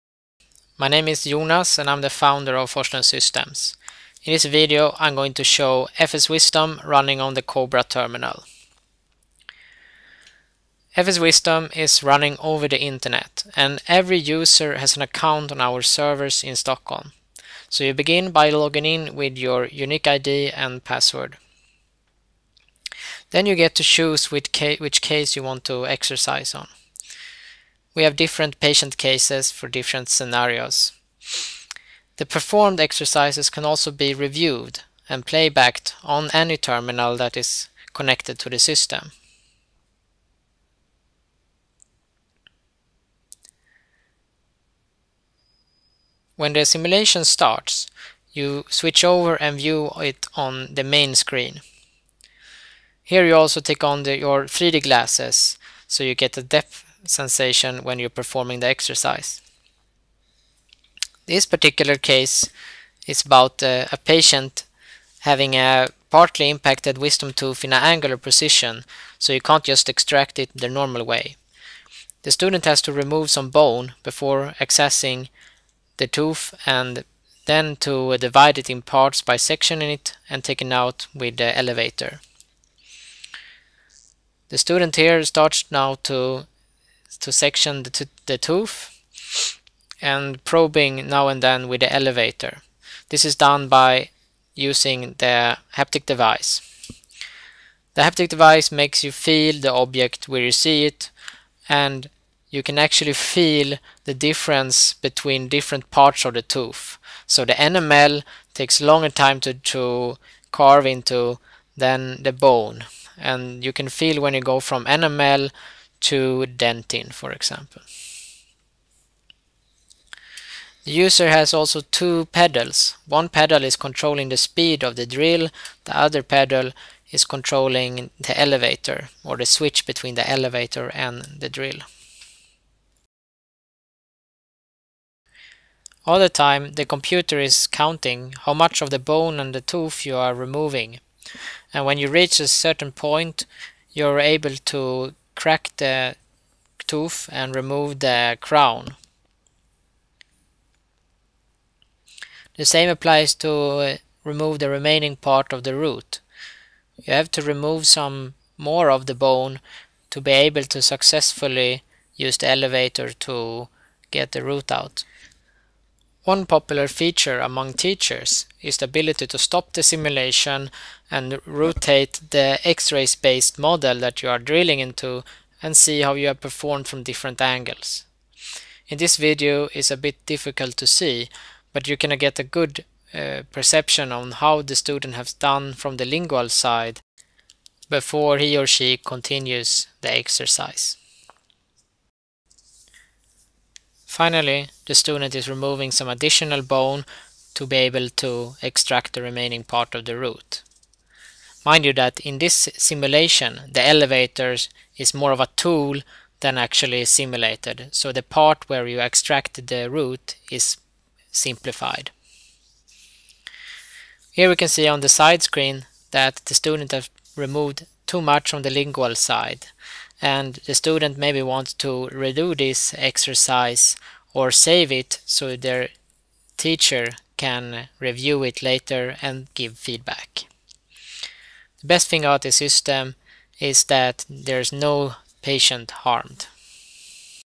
fs-wisdom
FS-Wisdom running on Kobra, a multimodal terminal with a haptic feedback device that lets the user feel the virtual jaw while carving in the bone and tooth.